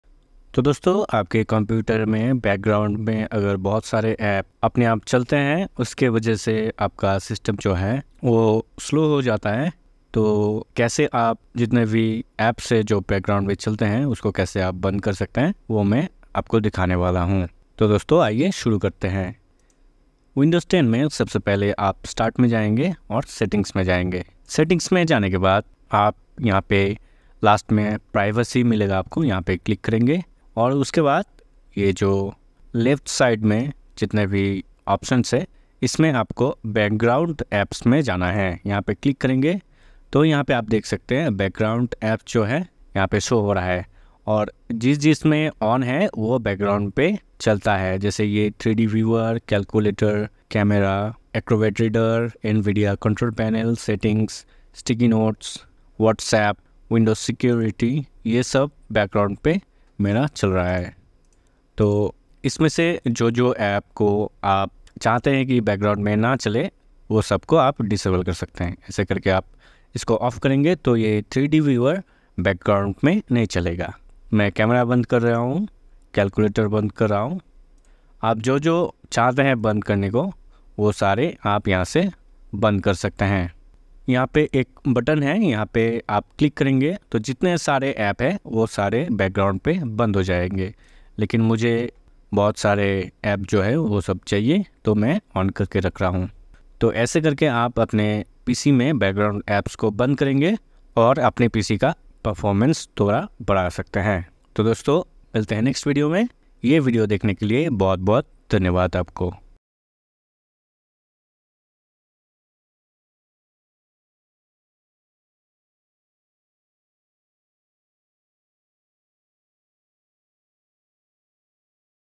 how to turn off background apps in windows 10 | Hindi
we'll guide you through the process of turning off background apps in Windows 10, helping you optimize system performance and conserve resources.

To begin, we'll show you the straightforward steps to access the background apps settings in Windows 10. From there, we'll explain the benefits of disabling background apps, such as improving system speed and extending battery life for laptop users.

Next, we'll demonstrate how to toggle off background apps individually or disable them altogether, depending on your preferences. We'll also provide insights into which apps are safe to disable and which ones you may want to keep running in the background.

By the end of this tutorial, you'll have the knowledge and tools to take control of your system's resource usage by effectively managing background apps in Windows 10.